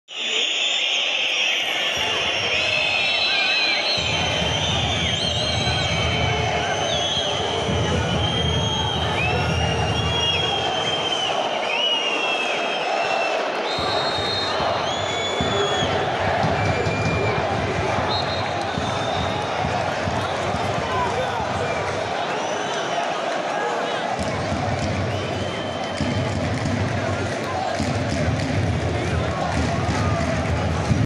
3gen Elazığspor – Silifke Belediyespor Karşılaşması Başladı
TFF 3. Lig 4. Grup karşılaşmalarında 3Gen Elazığspor, Silifke Belediyespor ile Elazığ Atatürk Stadyumu’nda karşılaşması başladı